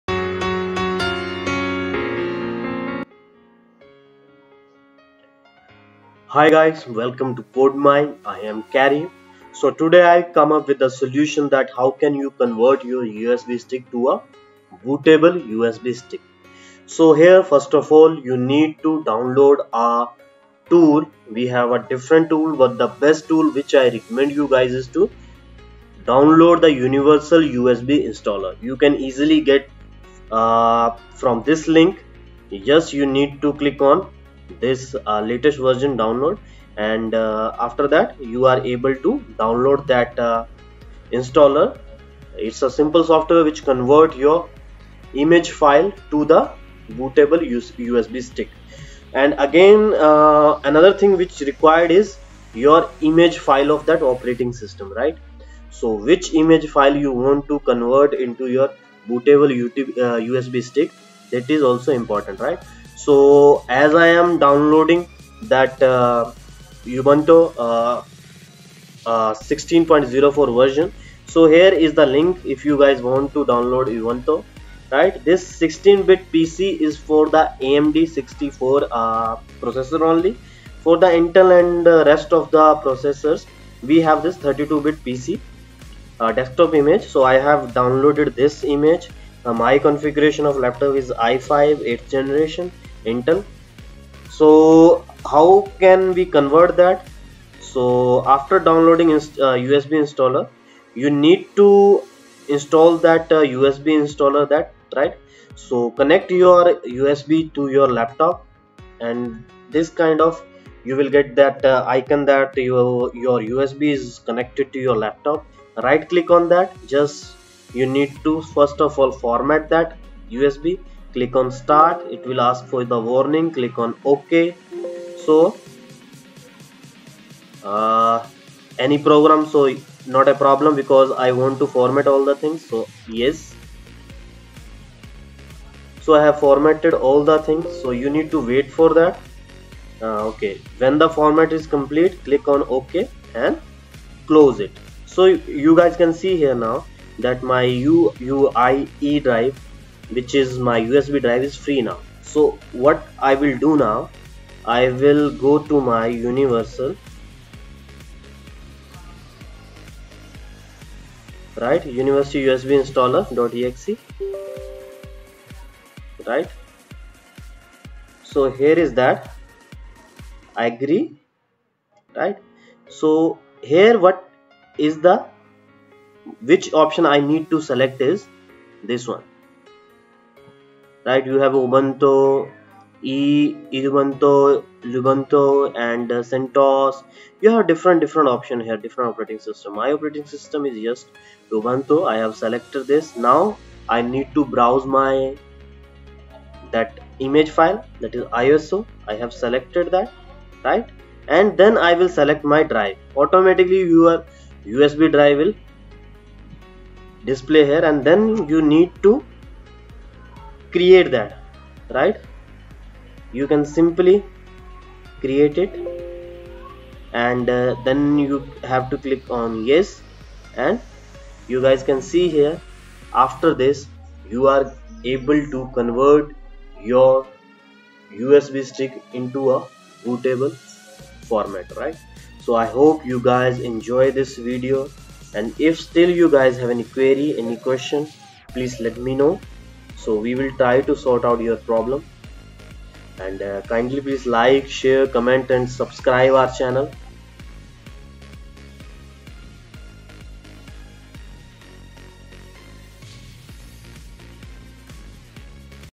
Bootable USB : Ubuntu installation, Windows Installation, Bootable pendrive
Here we have complete steps how to create bootable usb.

bootable pendrive mac,
bootable pendrive means,
bootable pen drive not working

Hi Guys
These are the steps to convert USB to Bootable USB.